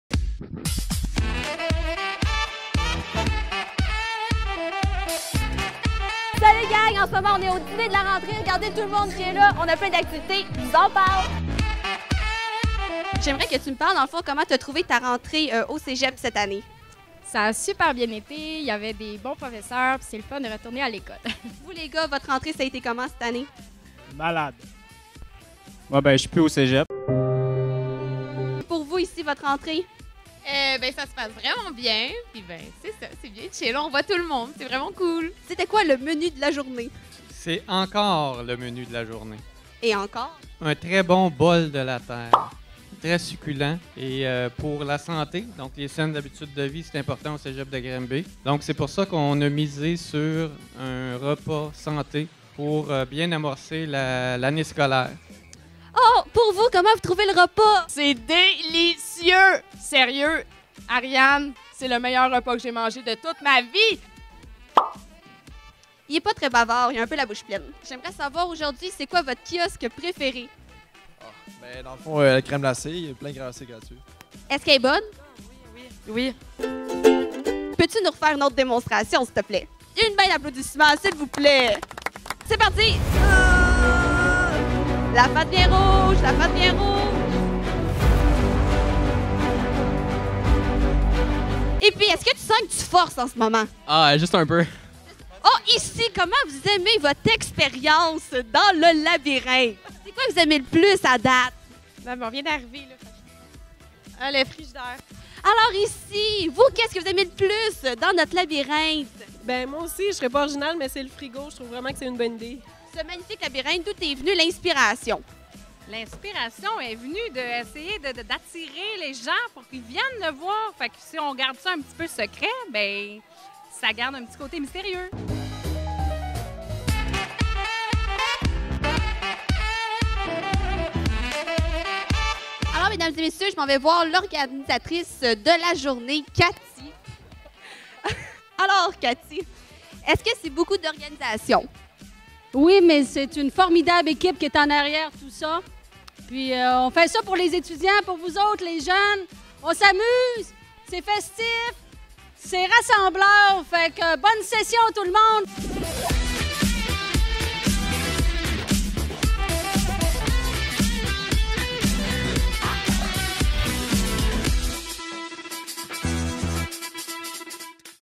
Fête de la rentrée 2018 - Cégep de Granby
Un dîner d'accueil écoresponsable et « zéro déchet » pour les étudiants du Cégep de Granby de l'automne 2018.